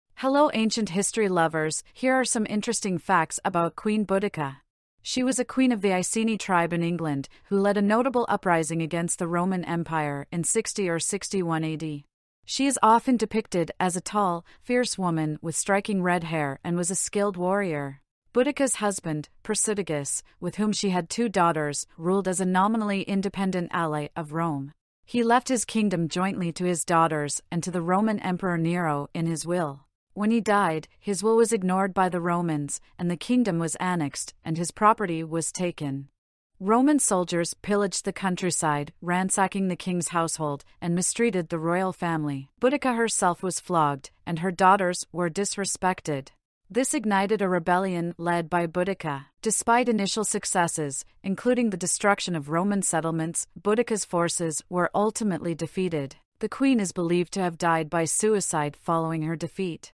Queen Boudica: Ancient Britain's Fierce Queen of War and Her Rebellion against Emperor Nero
With this video, Queen Boudica: Ancient Britain's Fierce Queen of War and Her Rebellion against Emperor Nero, 👑 dive into the heart of Ancient Britain with "Age-old Worlds" as we unravel the tale of Queen Boudica, the indomitable warrior queen of the Iceni tribe. 🛡️ Witness her courageous uprising against the mighty Roman Empire and Emperor Nero, a saga of bravery, betrayal, and a quest for justice. From her strategic battles to the tragic end, discover the legacy of a queen who became a symbol of resistance.